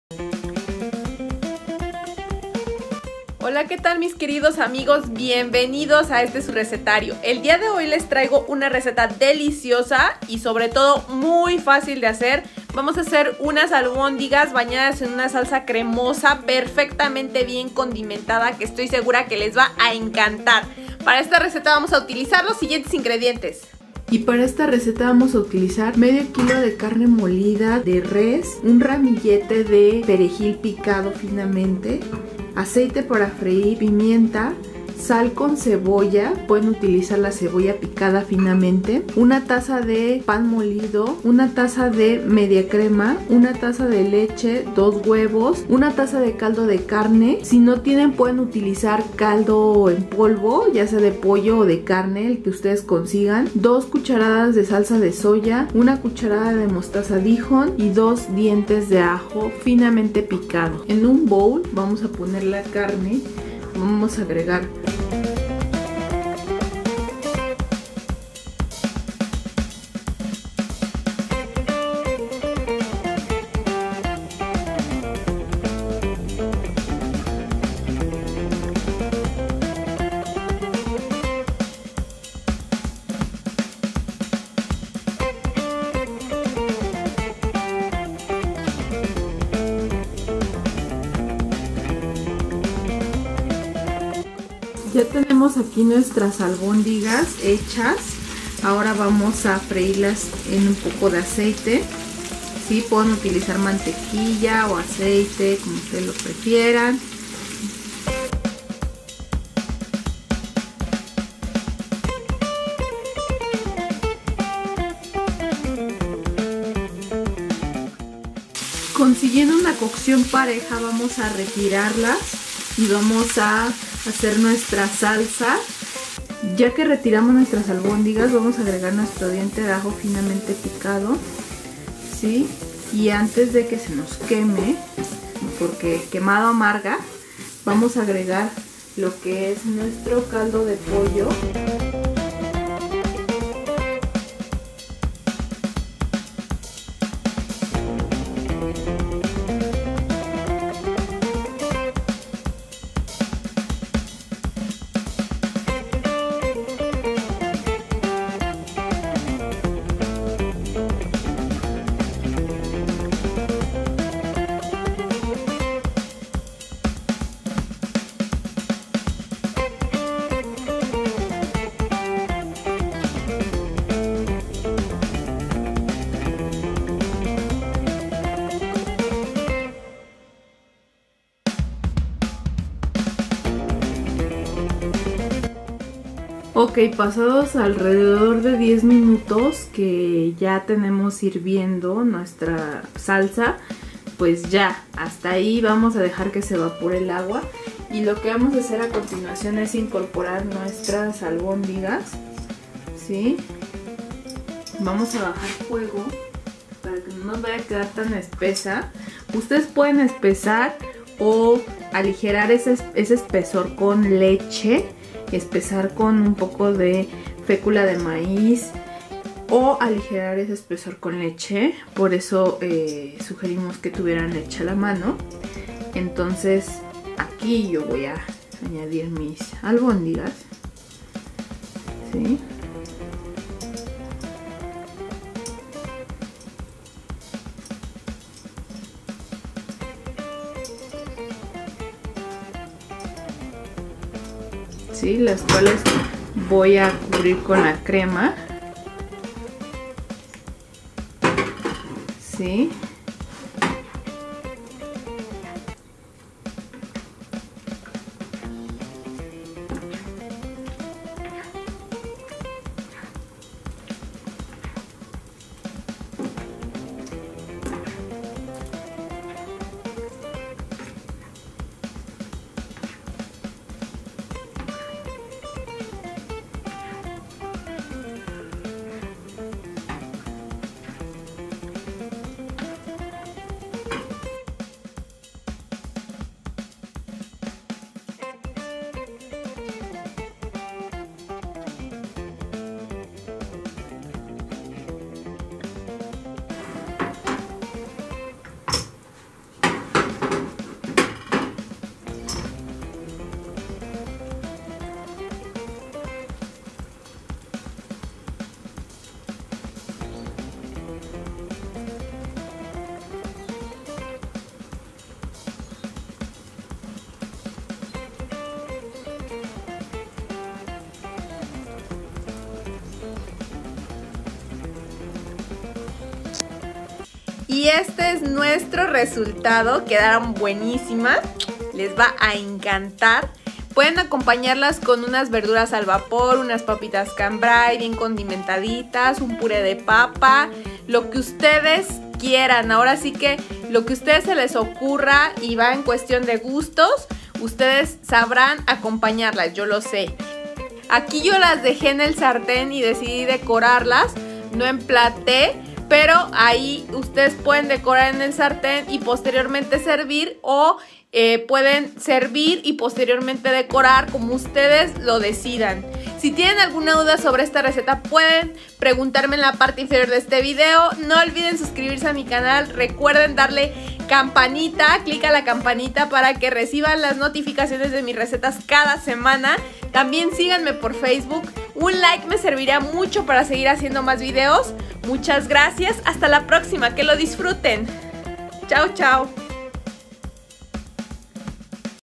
Albondigas en ESPECIA CREMOSA
Albondigas de ternera bañadas en una salsa cremosa con un toque de mostaza, perfectamente condimentada. Simplemente DELICIOSA!!!